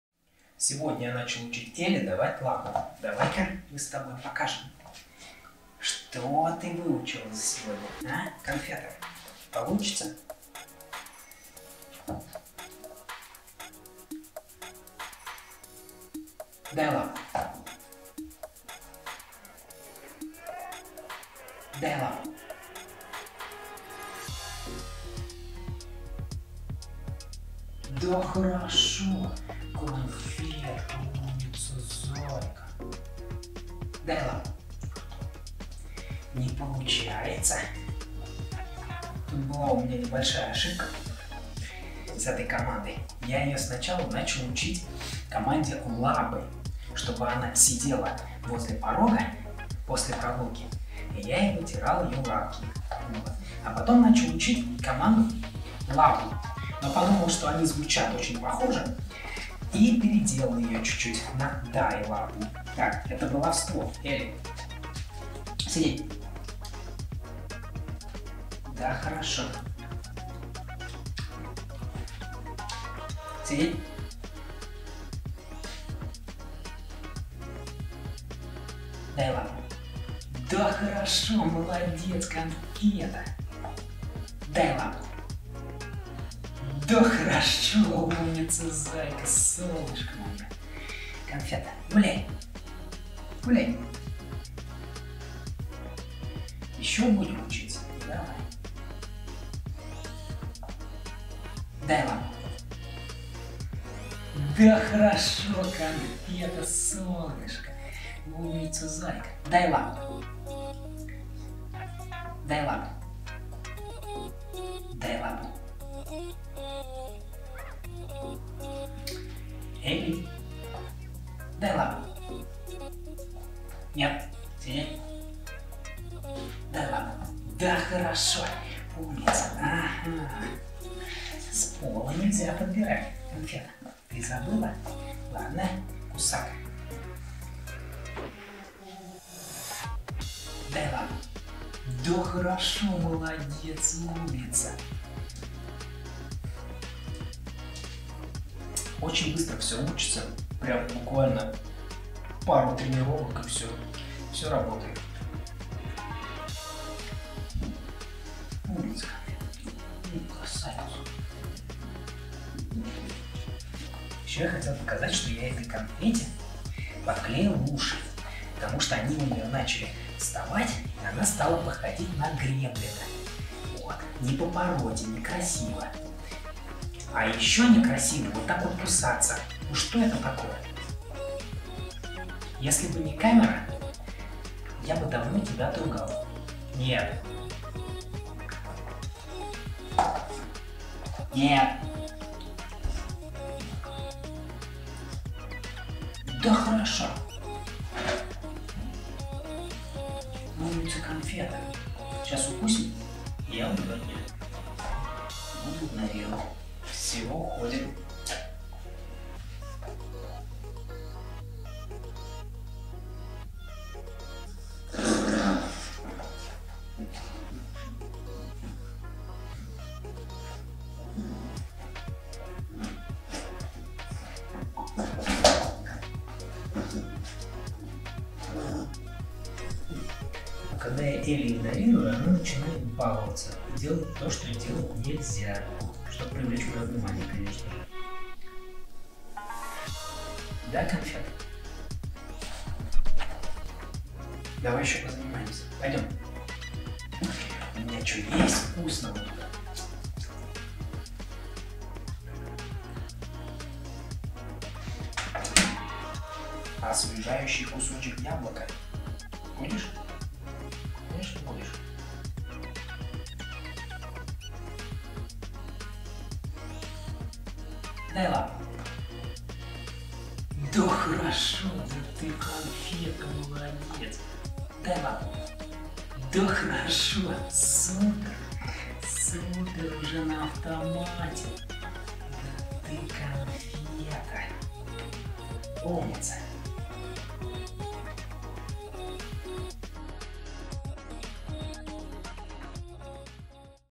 Воспитание щенка. Часть II
Воспитываю щенка породы стаффордширский бультерьер (стафф, стаффбуль). Показываю как мы с Элли учим команды, как играем, с какими проблемами сталкиваемся. Видео будут интересны желающим завести щенка и просто людям, любящим собак.